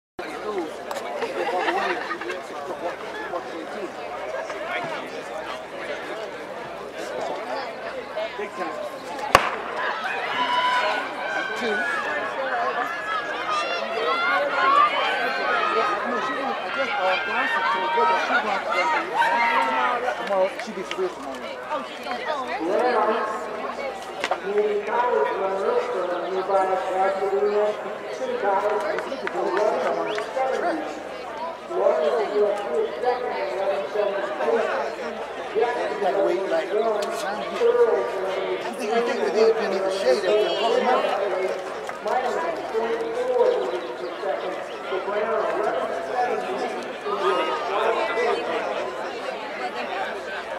Women 100 meter Dash Section 2 Open/Unattached Mt. SAC Relays 2013
Section  2     Wind: -0.4
  1 Omhunique Browne             Pasadena CC              11.70
  2 Loudia Laarman               USC                      11.76
  3 LaQuisha Jackson             San Diego St.            11.81
  4 Angelica Earls               Nevada                   11.83
  5 LaTisha Holden               Unattached               11.88
  6 Kailey Bostic                UT-Arlington             12.01
  7 Kelly Fogarty                Team Movin Shoes         12.02
  8 Meritzer Williams            Unattached               12.04
  9 Brittney Banister            Grand Valley St.         12.24